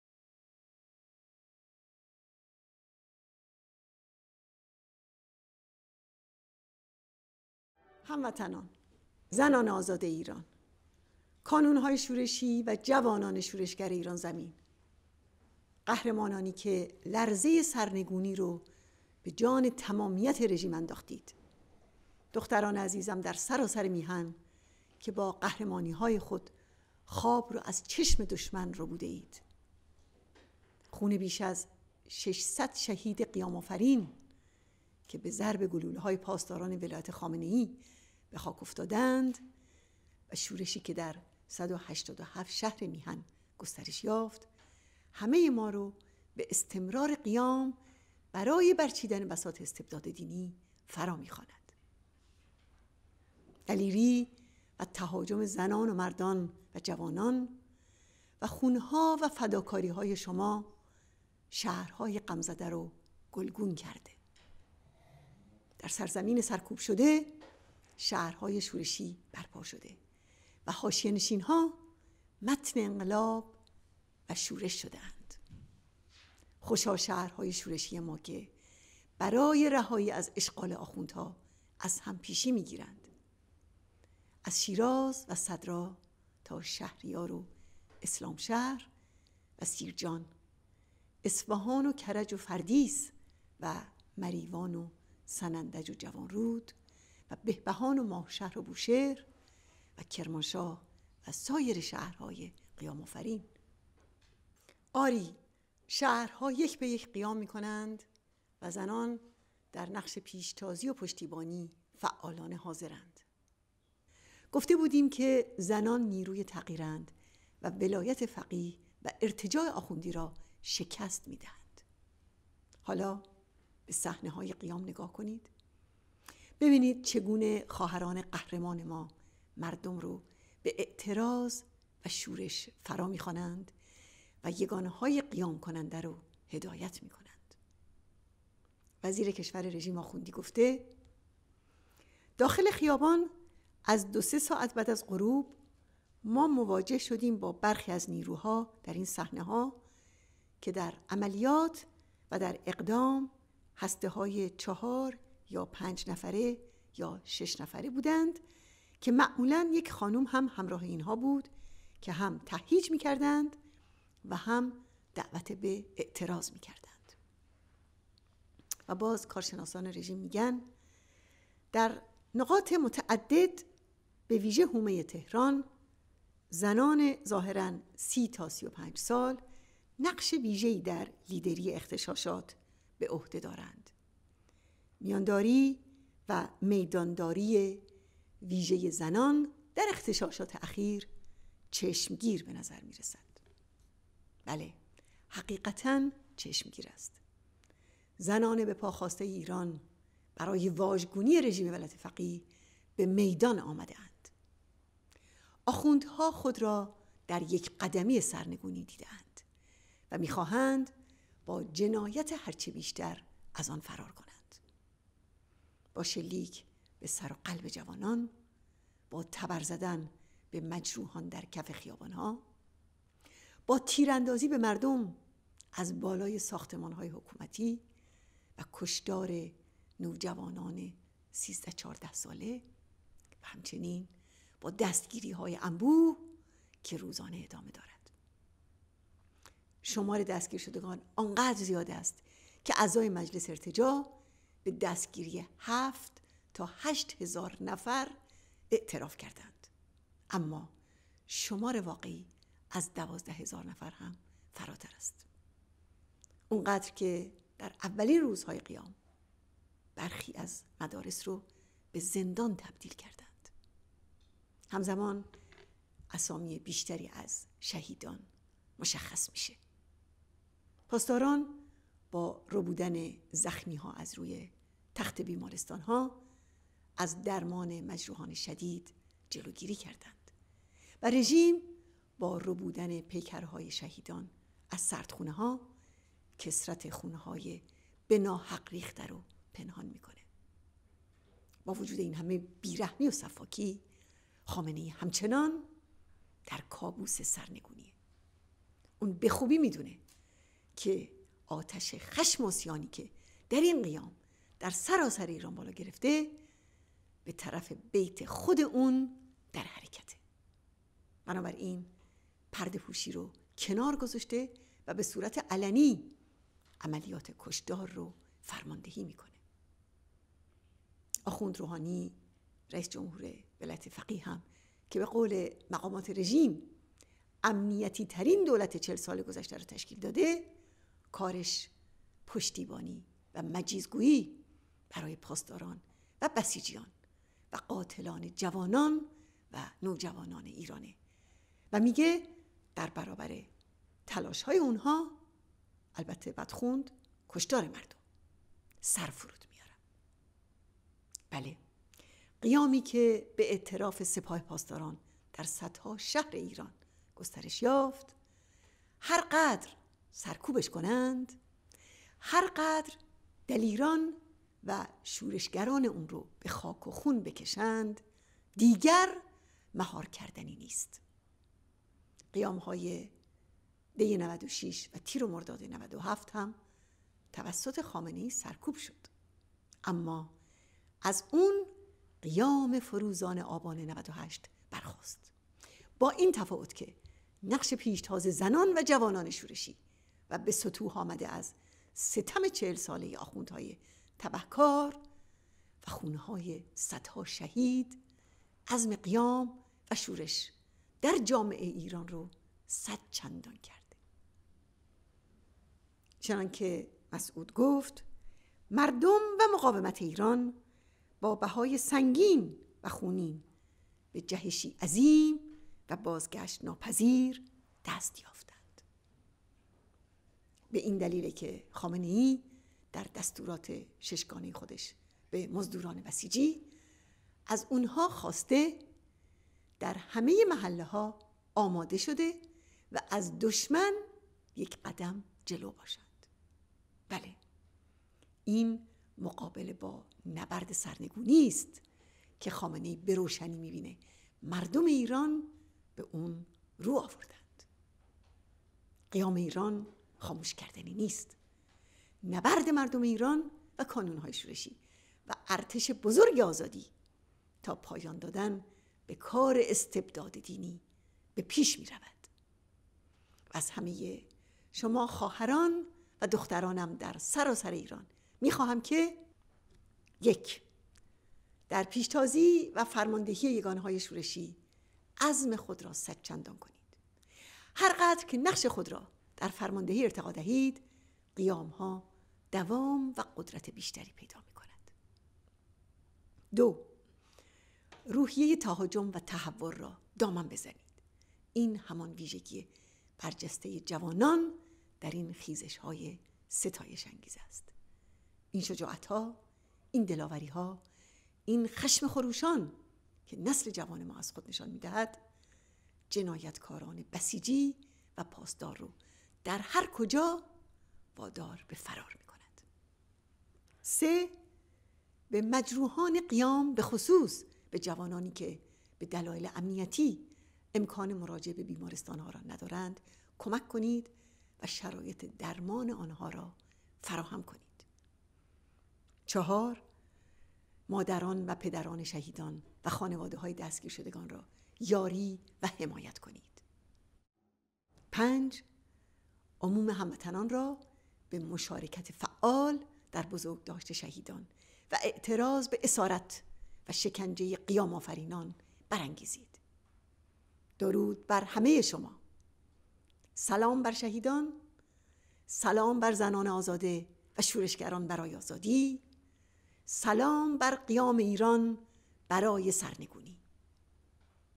Maryam Rajavi - Iran uprising- Women are the force for change
Heroes who have shaken the regime to its foundations,
My dear daughters who have infuriated the enemy with your heroism,
The blood of more than 600 martyred protesters shot to death by Khamenei’s revolutionary guards and the rebellion that expanded to 187 cities across the country inspire all of us to continue the uprising to topple the ruling religious fascism.
The courage exhibited by women and men and young people, and your sacrifices, have affected the grieving cities. Rebellious cities have risen up in our fettered nation and shanty towns have become hotbeds of revolution and rebellion.